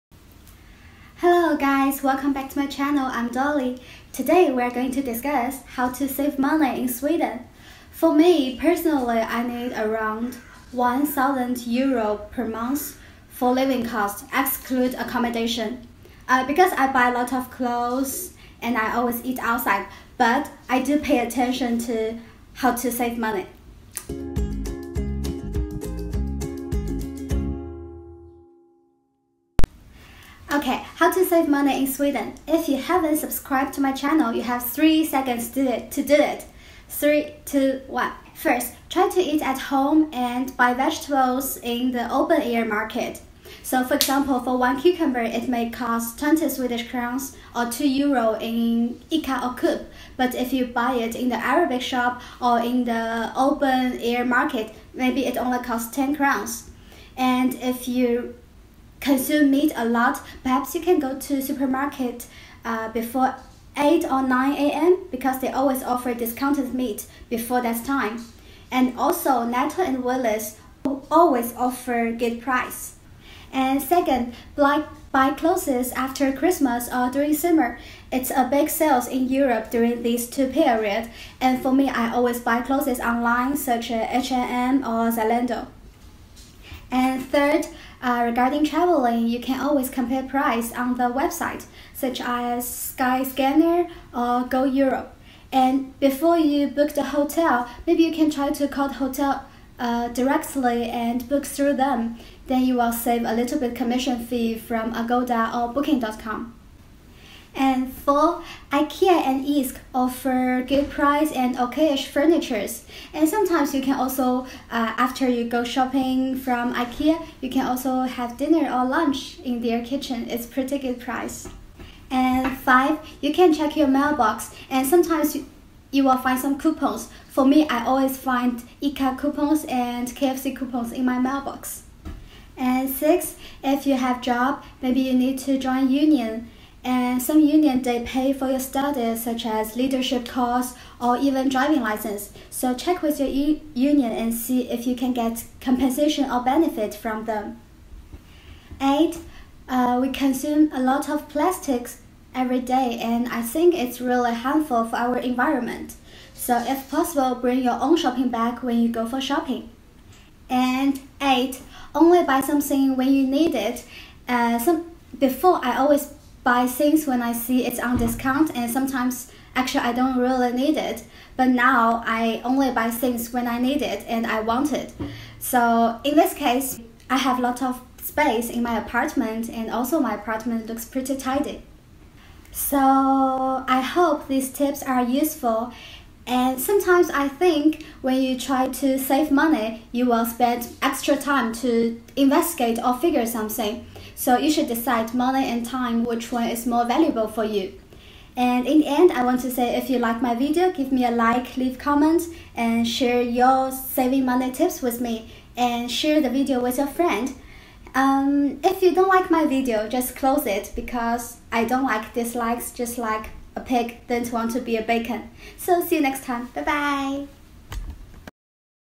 How to save money in Sweden? / Hej! I am Dolly/ 8s aving cost tips
Today we are going to discuss how to save money in Sweden.

Sometimes you think you save money but actually, you spend extra time. It's up to you to decide time and money, which one is more valuable for you.

If you don’t like it, just close it. DON’T press “dislike”. I hate “dislike” just like a pig hates to be bacon.